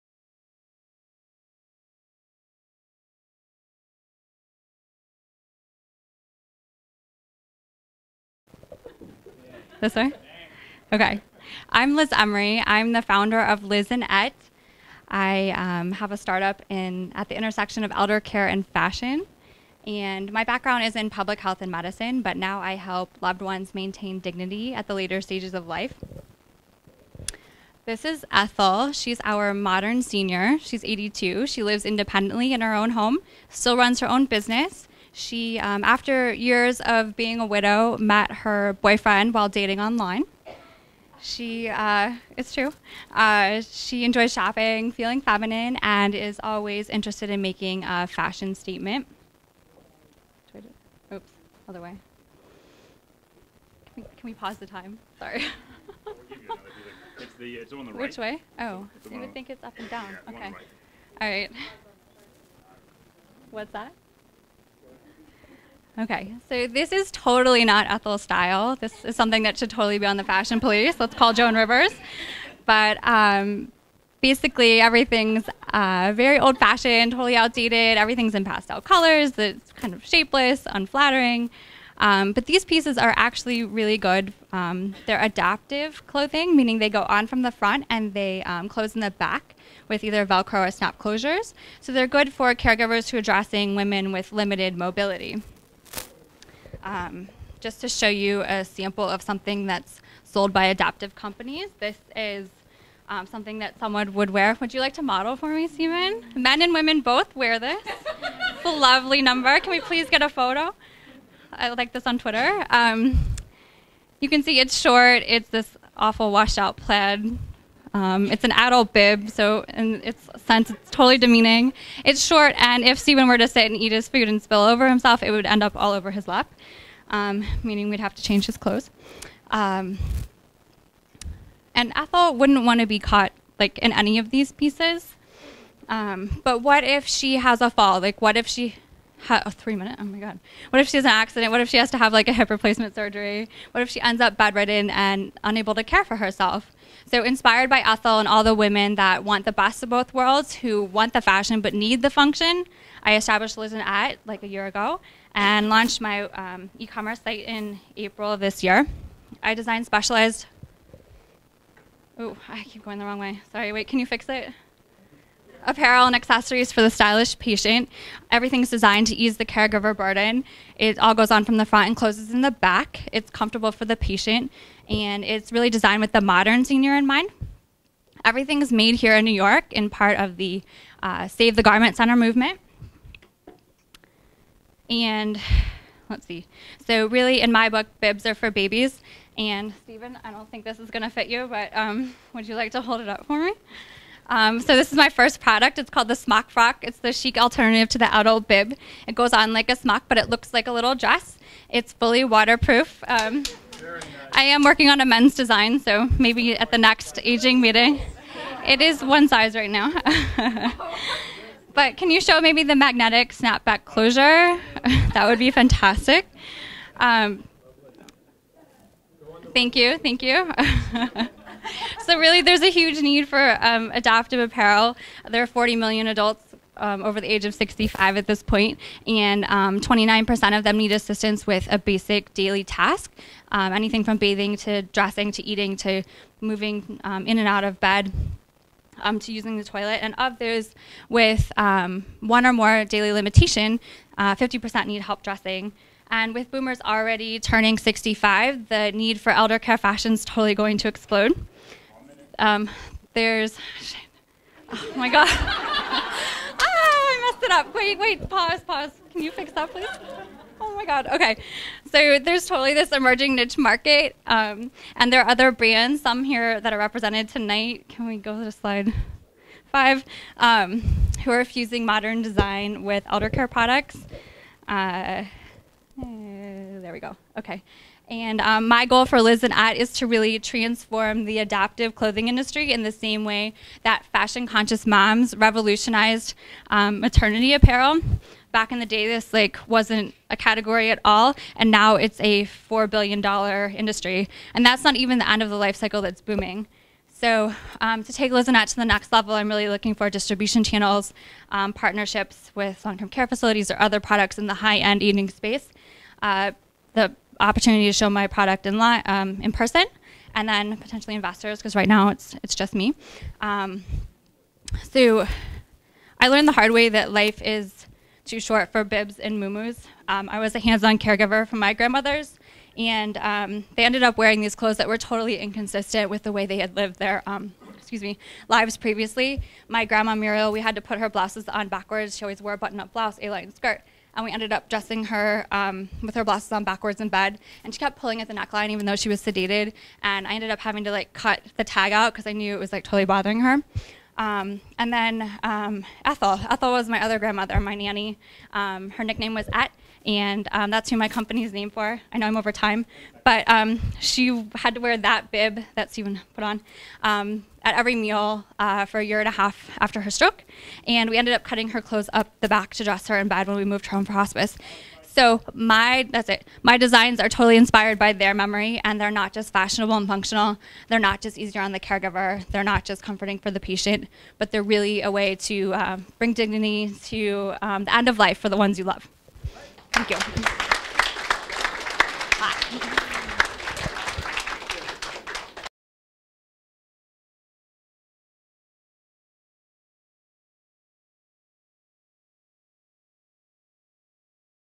A2 | NYC (12-08-28) Liz&Ett
Liz Emery from Liz And Ett presents at the Aging2.0 | New York meeting, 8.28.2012